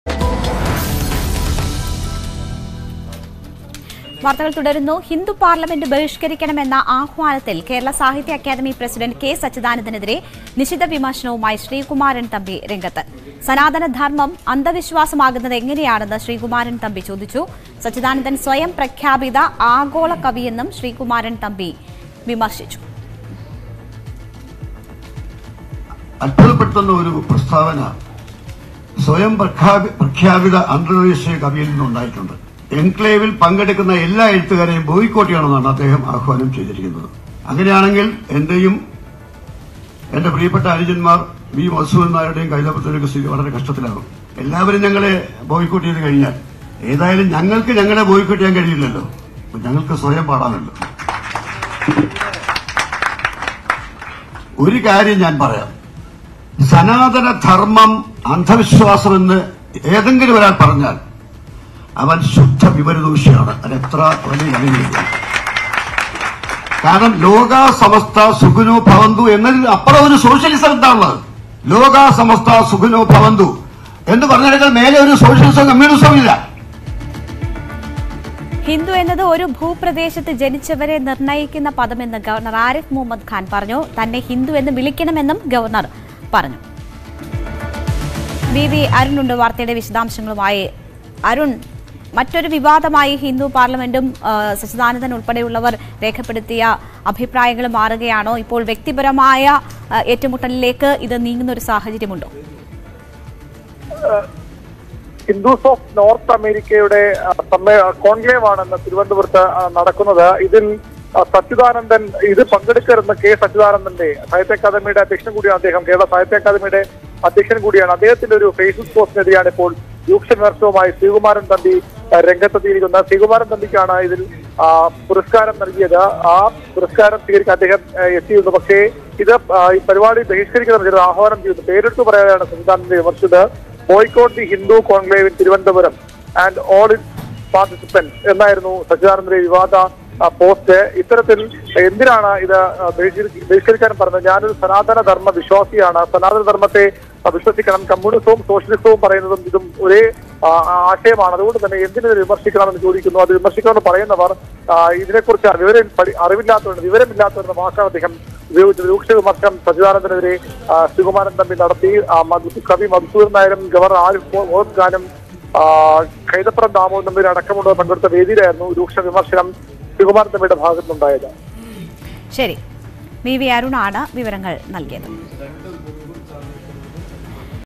"K Satchidanandan സ്വയം പ്രഖ്യാപിത ആഗോള കവി"; പരിഹാസവുമായി Sreekumaran Thampi | Kerala News
K Satchidanandanനെതിരേ നിശിത വിമർശനവുമായി Sreekumaran Thampi.ഹിന്ദു കോൺക്ലേവ്  ബഹിഷ്കരിക്കണമെന്ന ആഹ്വാനത്തിനാണ് മറുപടി നൽകിയിരിക്കുന്നത് .സനാതന ധർമ്മം അന്ധ വിശ്വാസം ആകുന്നത് എങ്ങനെയാണെന്നും K Satchidanandan സ്വയം പ്രഖ്യാപിത ആഗോള കവിയെന്നും  ശ്രീകുമാരൻ തമ്പി വിമർശിച്ചു.